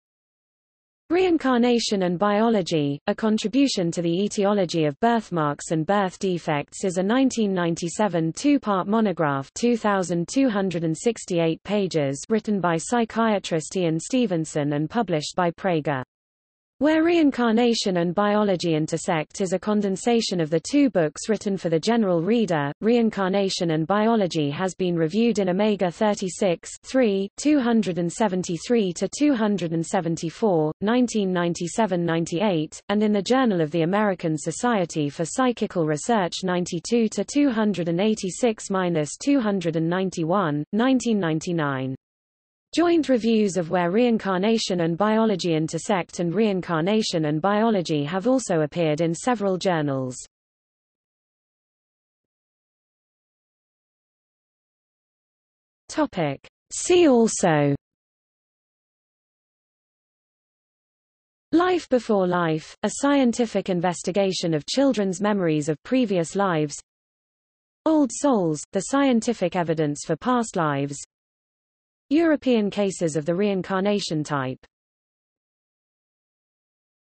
Reincarnation and Biology | Wikipedia audio article
00:00:52 See also

- Socrates

SUMMARY
Reincarnation and Biology: A Contribution to the Etiology of Birthmarks and Birth Defects is a 1997 two-part monograph  (2268 pages) written by psychiatrist Ian Stevenson and published by Praeger. Where Reincarnation and Biology Intersect is a condensation of the two books written for the general reader.Reincarnation and Biology has been reviewed in Omega 36(3):273-274, 1997–98, and in the Journal of the American Society for Psychical Research 92:286-291, 1999. Joint reviews of Where Reincarnation and Biology Intersect and Reincarnation and Biology have also appeared in several journals.